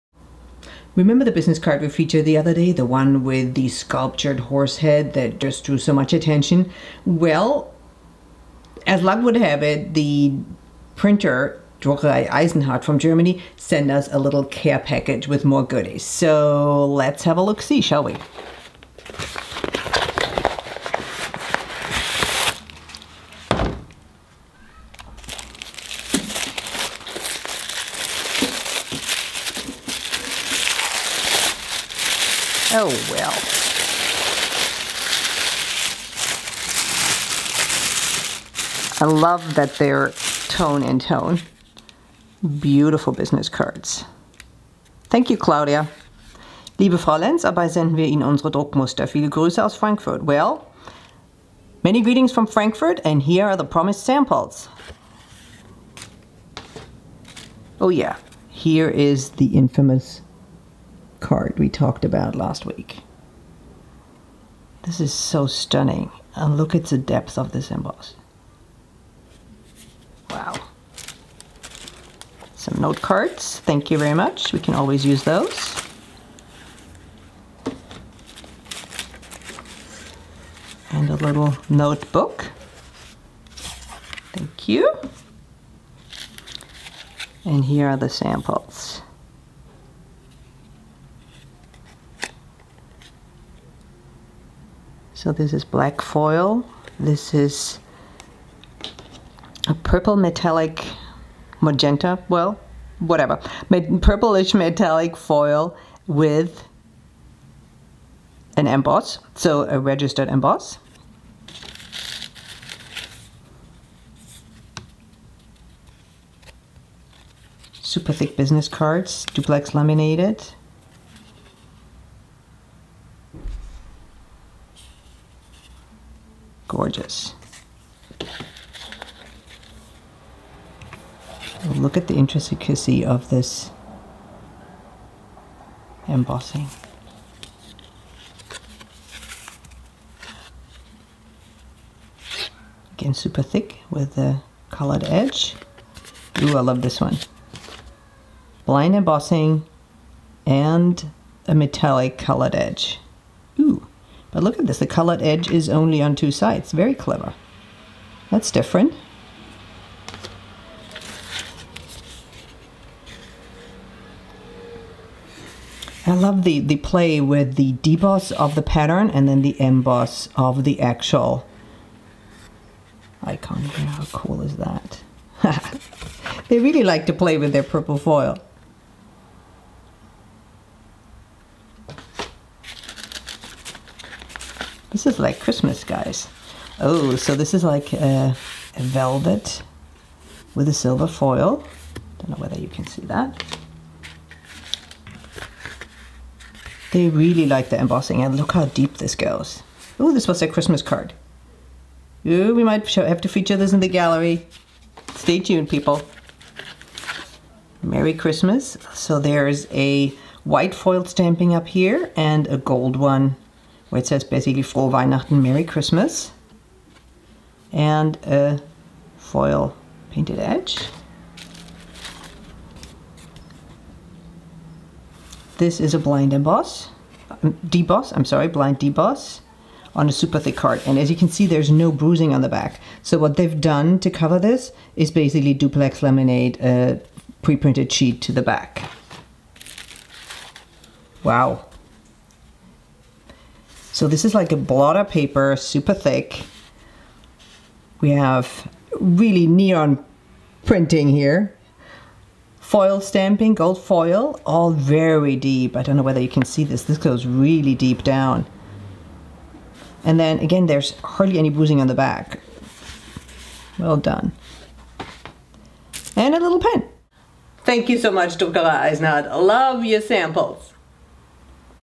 PaperSpecs | Unboxing stunning print samples from Germany
When the best of the best share their samples, we cannot help but ooh and ahh... Join PaperSpecs founder Sabine Lenz when she opens a care package from German Druckerei Eisenhardt.